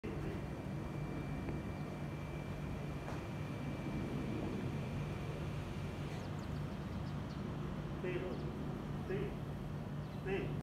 Thai VietJetAir Airbus A320-214 HS-VKF flyover
Thai VietJetAir Airbus A320-214 (A320) HS-VKF flyover in Bangkok (VZ108 BKK-CNX)